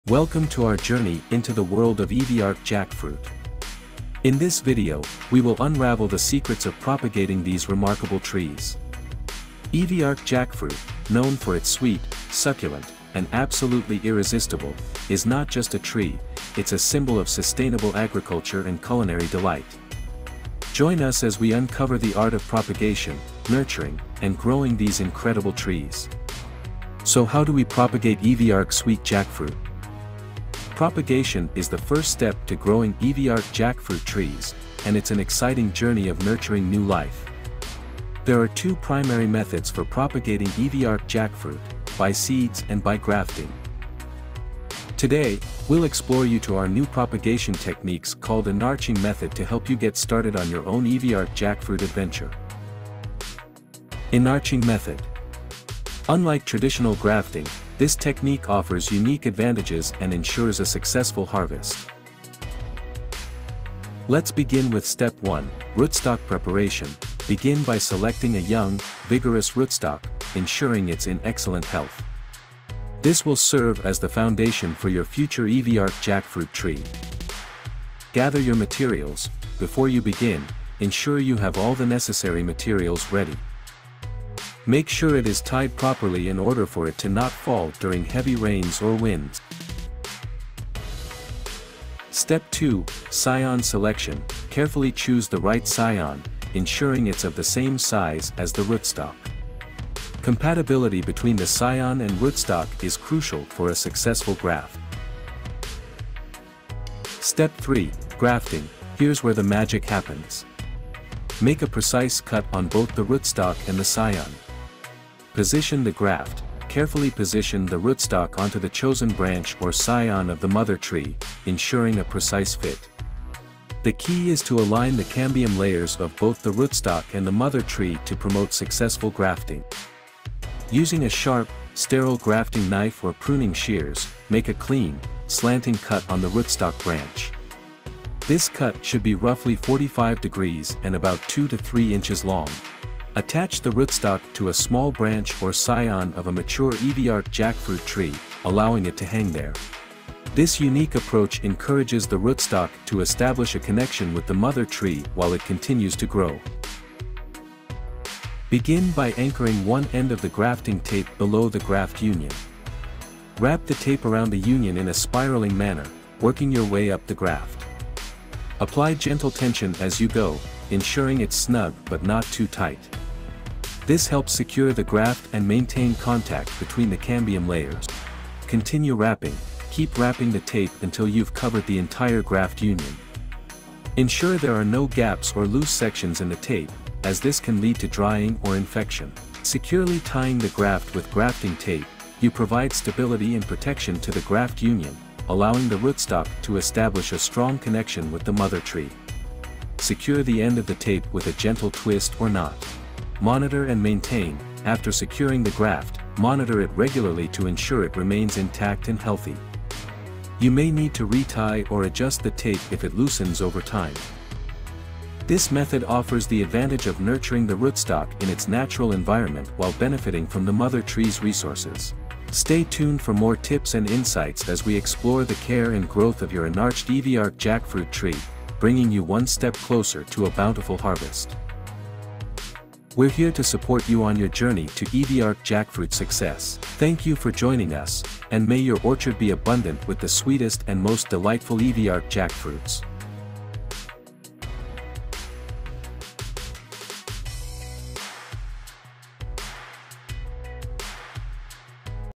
Eviarc Jackfruit - Discover the new way to propagate using Inarching method.
Apologies for the wrong number provided in this video. Here is the correct one #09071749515.

This video is about tutorial of how to graft a jackfruit using inarching method. Hope that you learn from this. 😊